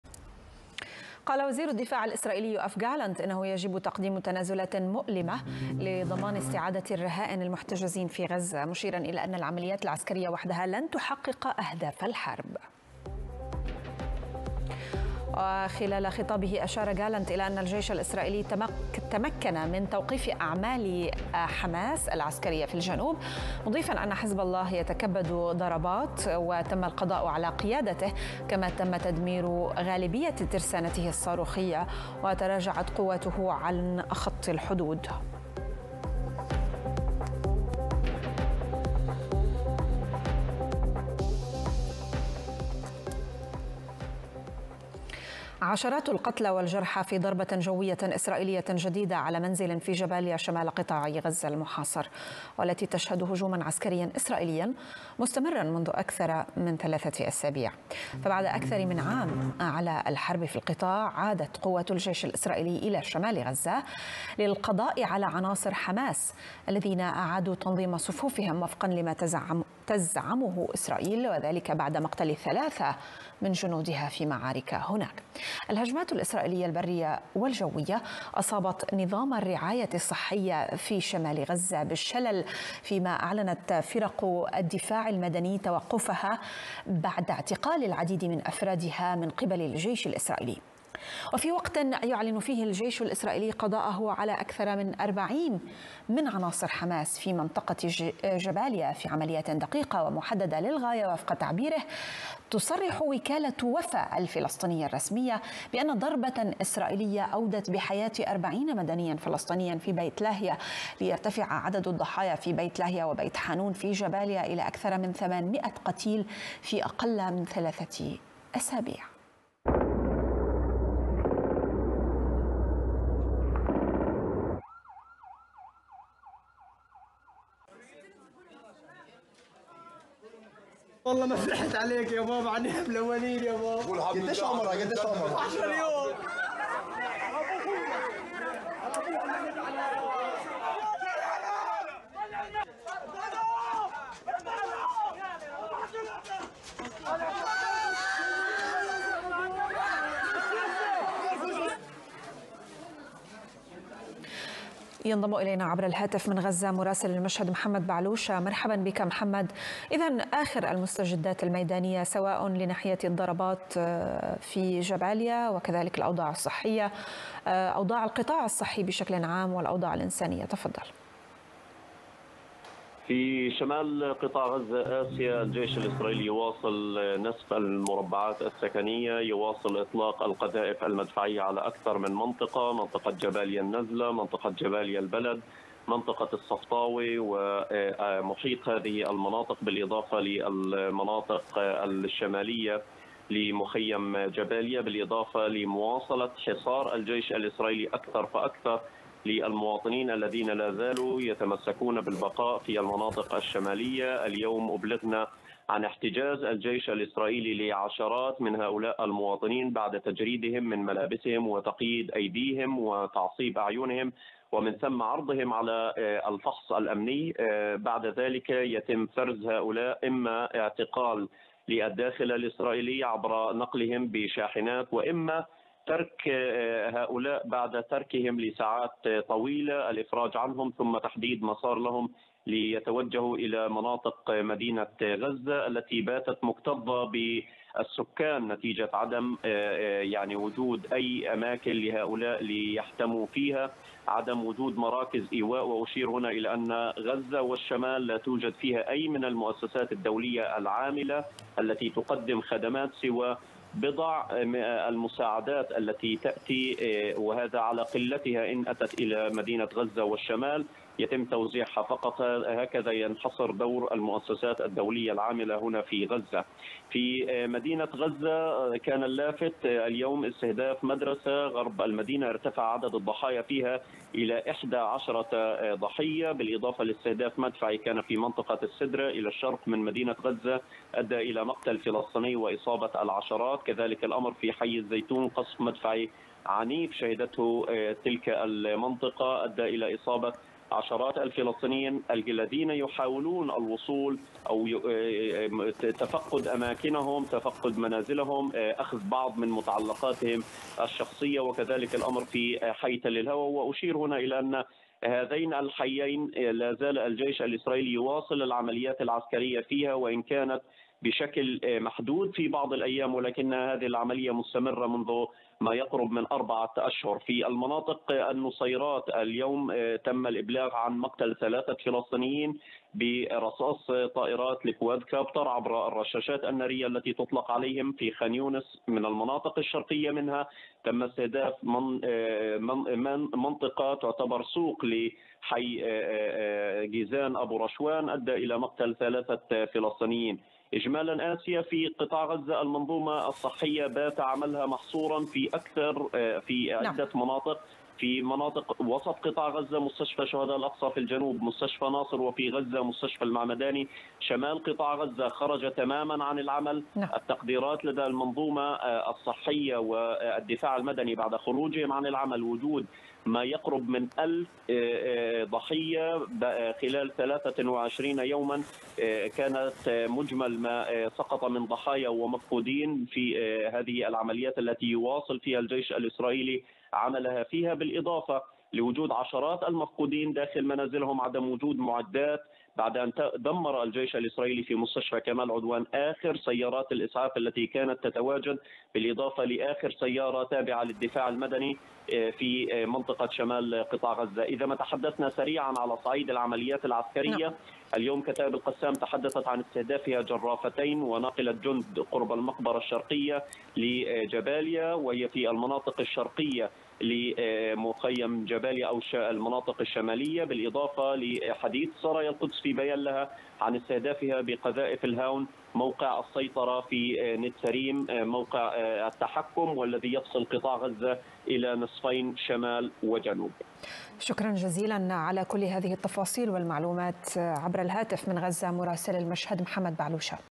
مراسل "المشهد" في غزة: الجيش الإسرائيلي يحتجز مئات من الشبان الغزيين
في هذه التغطية المباشرة للحرب الإسرائيلية على كل من اللبنان وغزة تستضيف الزميلة آسيا هشام مراسل "المشهد" في قطاع غزة الزميل محمد بعلوشة للحديث عن آخر التطورات للحرب على شمال وجنوب القطاع وأخر والوضع الصحي لسكان القطاع.